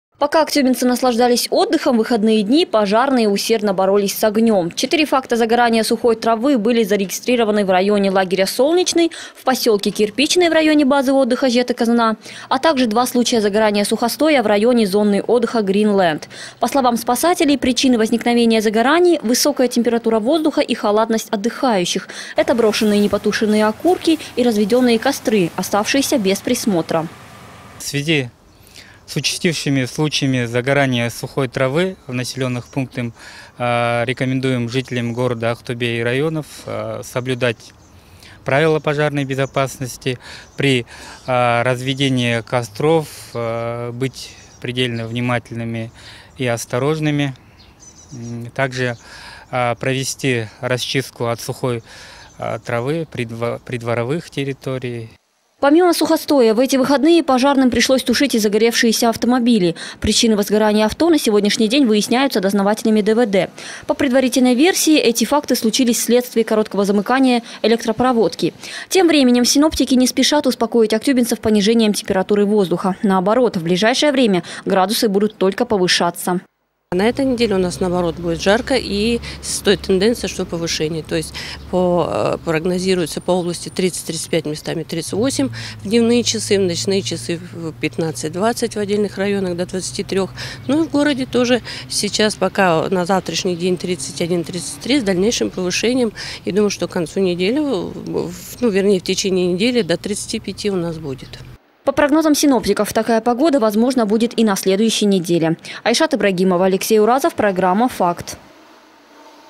В выходные горел сухостой в лагерях и зонах отдыха
В эти выходные пожарные боролись с огнём: факты загорания сухой травы были зарегистрированы в районе лагеря «Солнечный», в посёлке Кирпичный (в районе базы отдыха «Жети казына»), а также в районе зоны отдыха «Green land». По словам спасателей, причины возникновения возгораний - халатное отношение отдыхающих. Это и брошенные непотушенные окурки, и разведённые костры, оставшиеся без присмотра.
Помимо сухостоя в эти выходные пожарные тушили и загоревшиеся автомобили. Причины возгорания машин выясняют дознаватели ДВД. По предварительной версии, «виновато» короткое замыкание электропроводки.